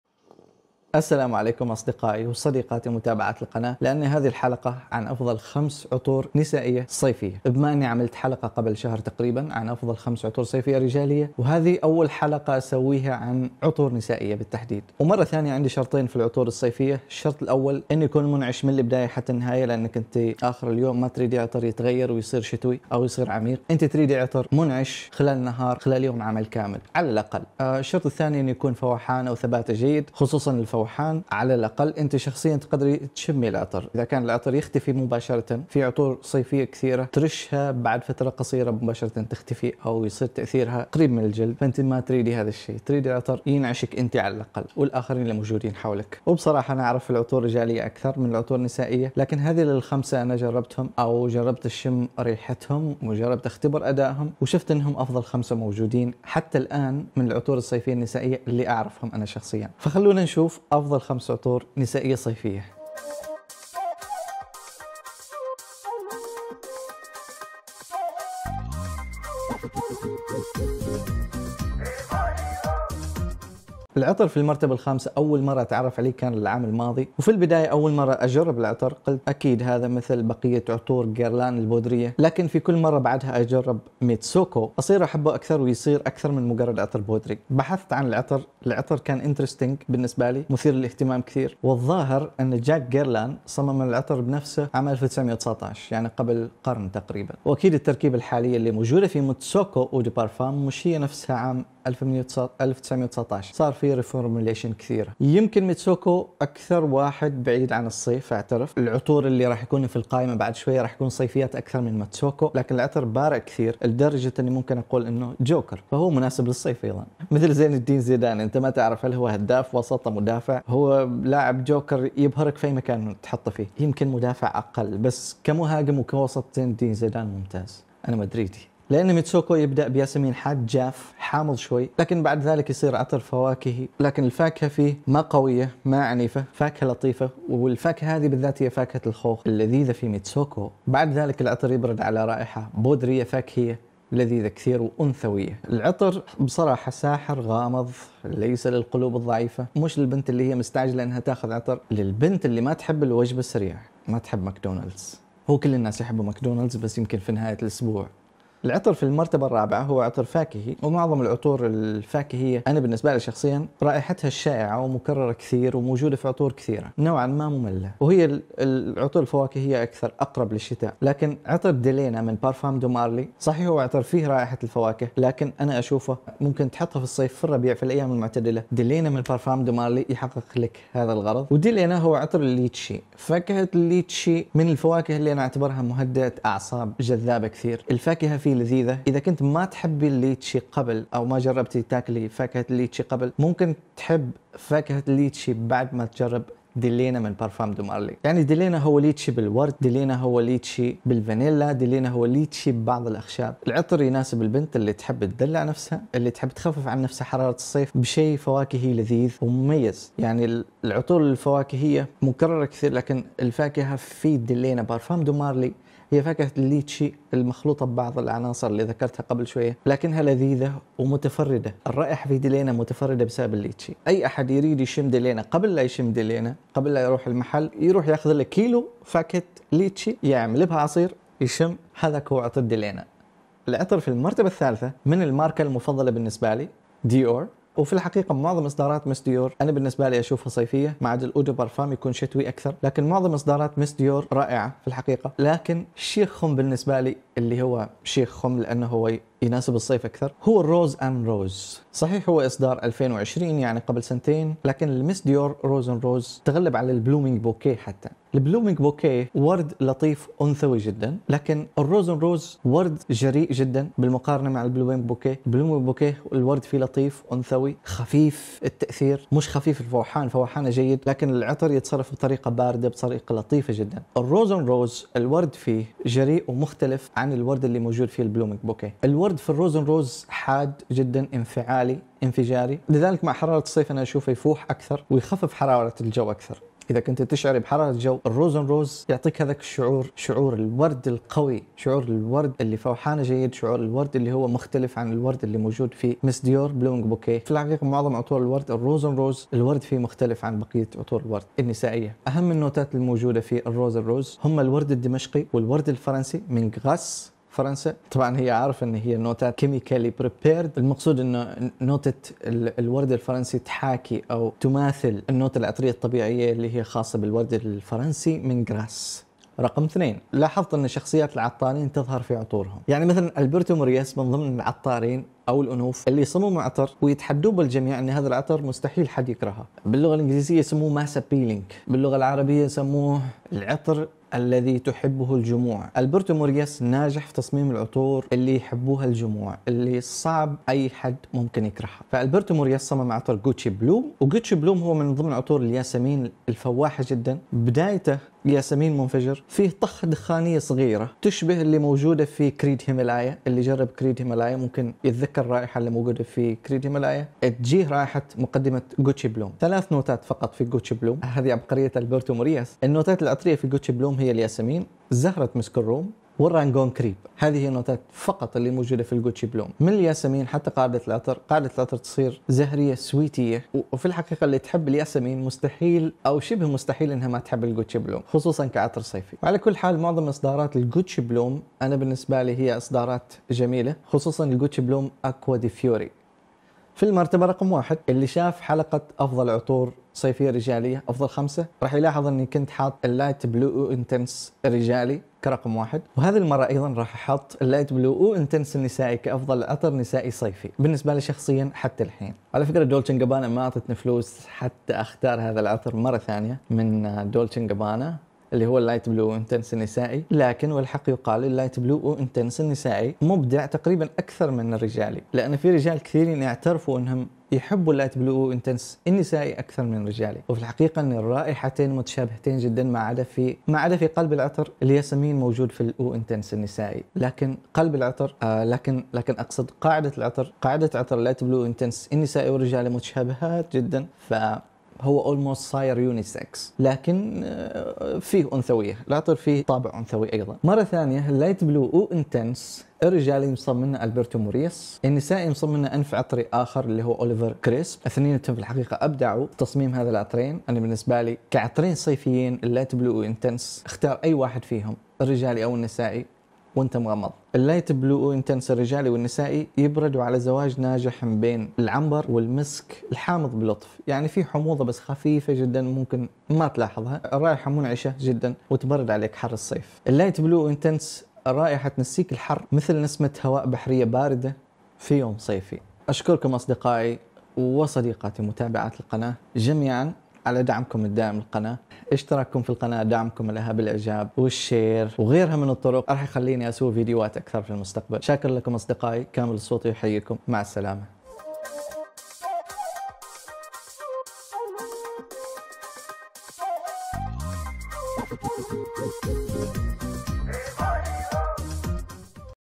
أجمل 5 عطور صيفية نسائية 2022 - Top 5 Summer Fragrances for Women 2022
السلام عليكم أصدقائي/صديقاتي متابعين القناة
حلقة اليوم لأجل حواء... أول حلقة عن عطور نسائية

الشكر الجزيل للمايسترو صابر عيسى صاحب موسيقى المقدمة
الشكر الجزيل لمصممة صورة الفيديو المصممة دلال السوطي

متابعة شيقة و أرجو أن تحب حواء الاختيارات في الحلقة